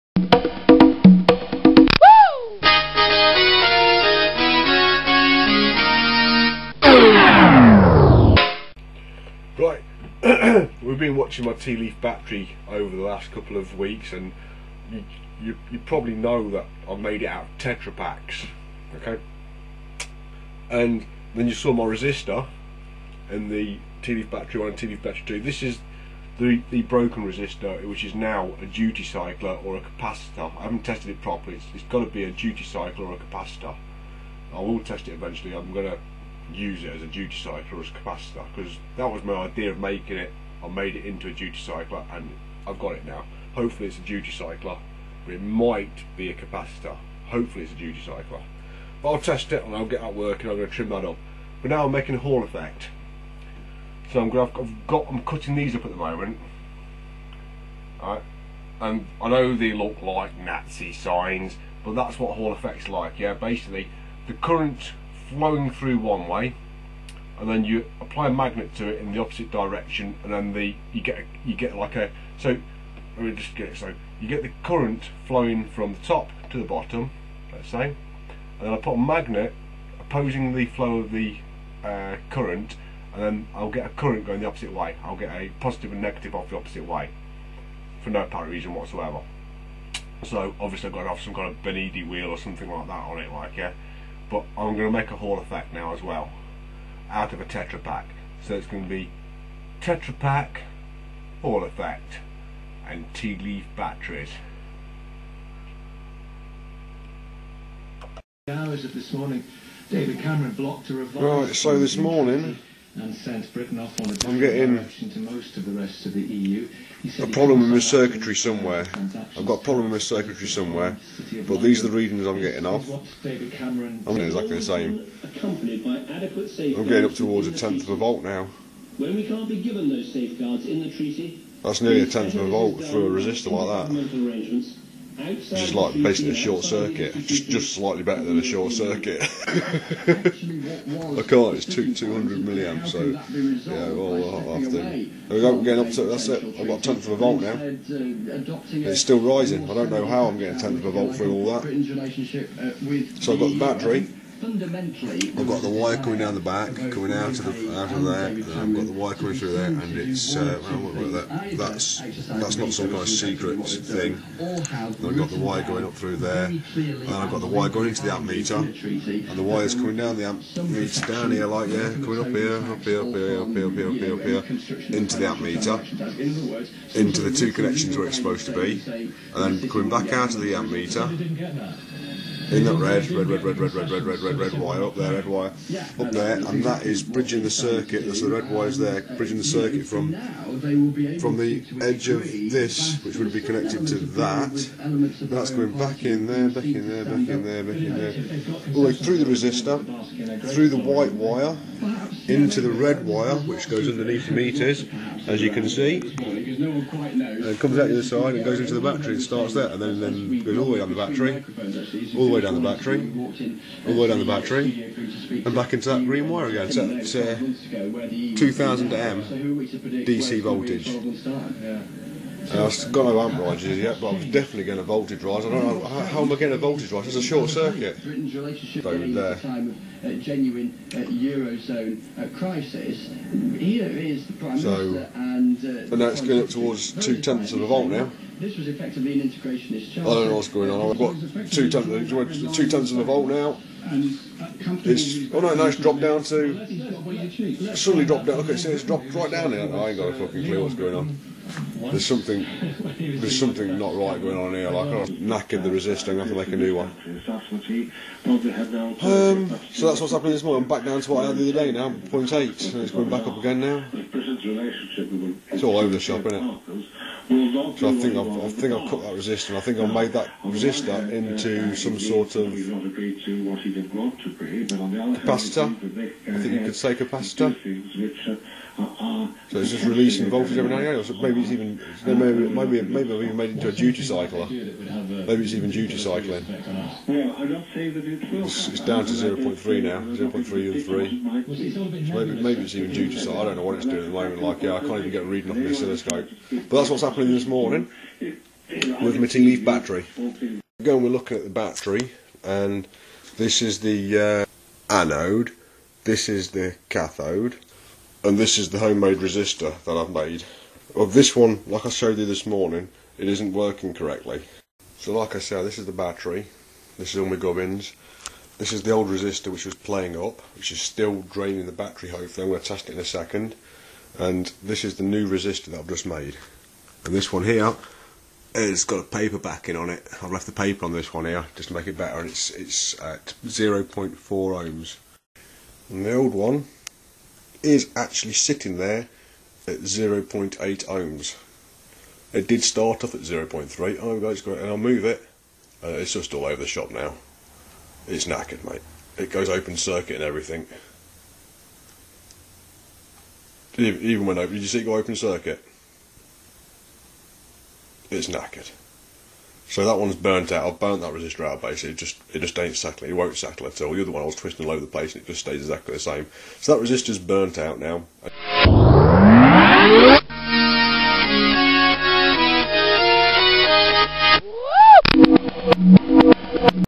Earth Batteries Tea Leafs HallEffect
The battery has been on load for 3 days. The effect being I have burnt out the first resistor.  The funny thing is that the soup packet is now acting like a capacitor or duty cycled circuit.

This effect is because the minor ionization in the short circuit is dragging mass along with the electrons flow and causing breaks in the circuit.  You can see this effect in toasters and other short circuit designs.  The elements become hardened as they gain or lose mass.  The same thing happens on a bigger scale in a nuclear cyclotrons when they enrich uranium.

So for you namby pamby idot CND freaks. Get rid of your toasters as they are causing radiation! And don't light fires as they are causing radiation. Oh no. There's nothing you can do!! Just let us make the technology and fuck off you fucking freaks!

Demonstrated nuclear enrichment of aluminum.